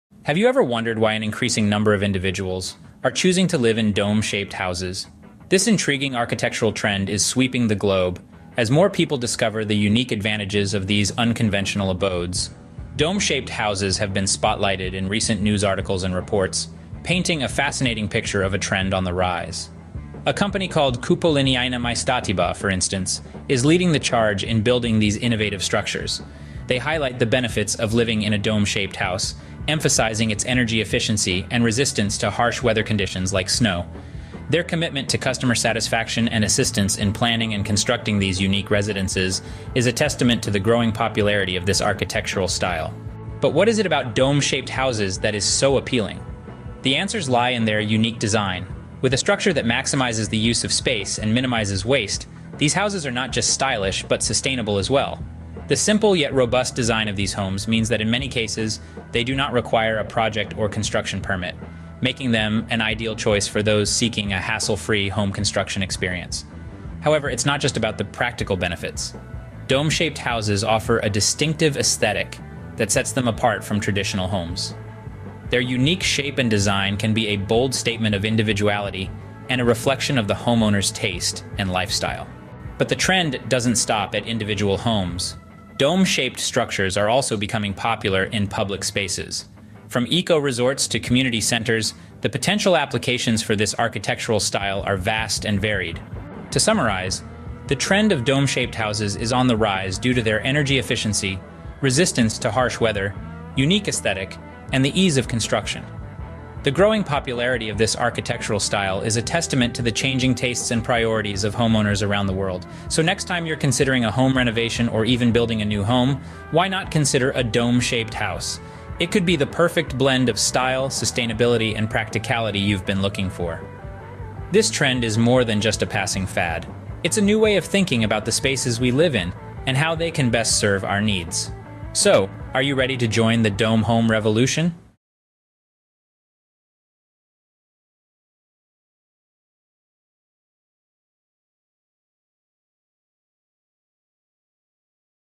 New technologies DOME HOUSES
Dome house technologies began to dictate new rules of construction and design. Dome houses, which are formed in 5 days, can be used as residential houses and as commercial premises. Fantasy can be realized with a dome-shaped house.